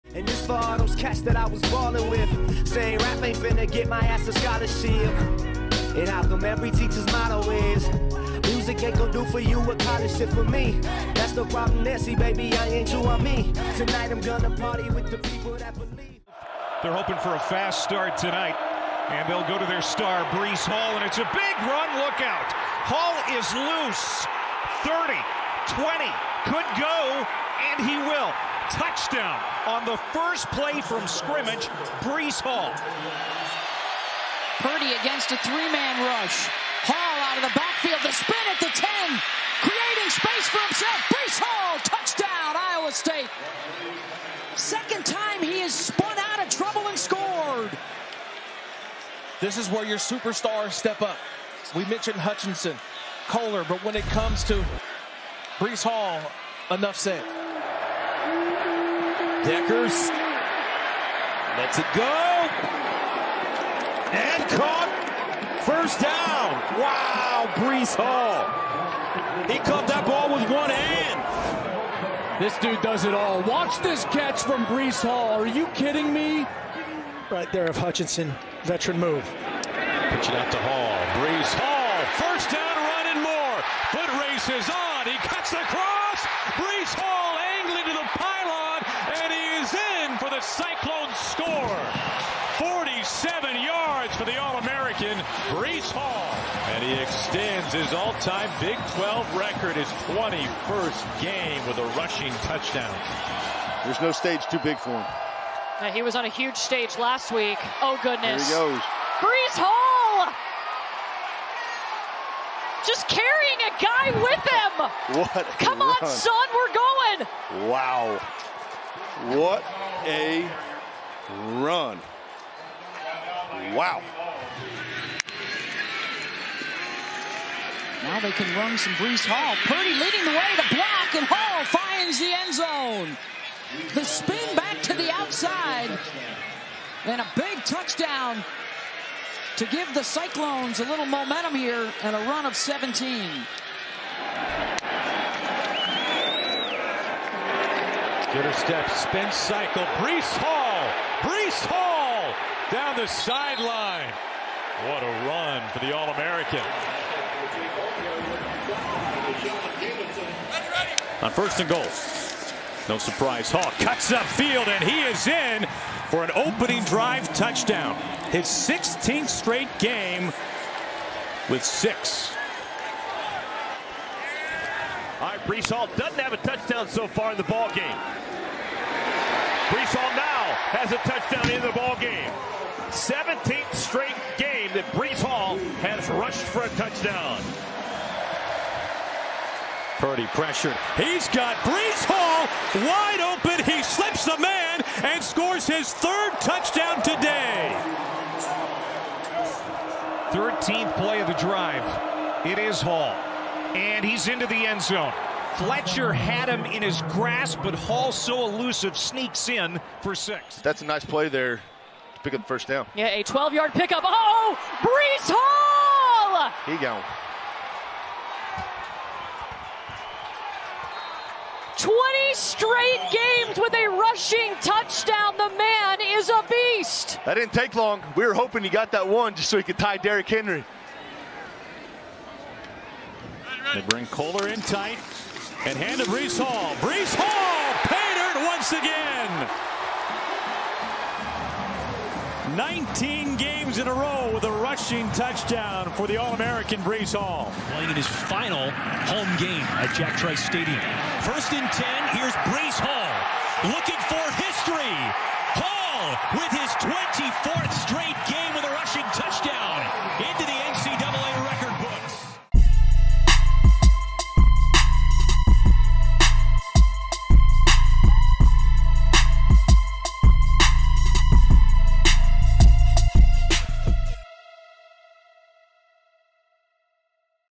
“Breece The BEAST” 😈 || Iowa State RB Breece Hall 2021 Highlights ᴴᴰ
Iowa State RB Breece Hall

Iowa State RB Breece Hall finished his career as one of the best running backs in college football history. In 2021, he was a Doak Walker finalist & Consensus All-American for the second year in a row. He also set a new NCAA record with a rushing TD in his 24th consecutive game. Along with Michigan State RB Kenneth Walker III, Hall could be the first RB selected & could possibly be drafted in the 1st round.